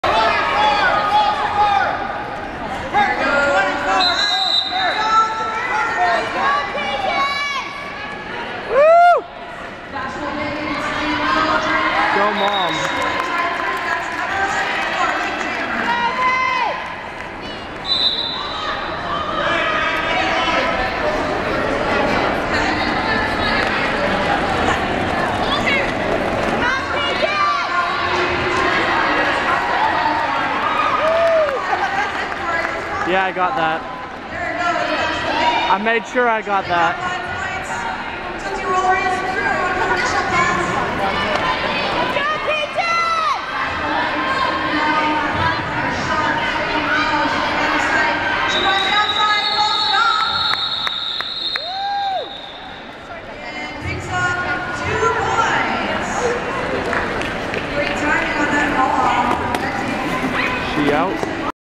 Roller derby 11-Emerald City Junior Gems
Grease monkeys vs. misfit mafia
02/2012
Score 72-9 in favor of Monkeys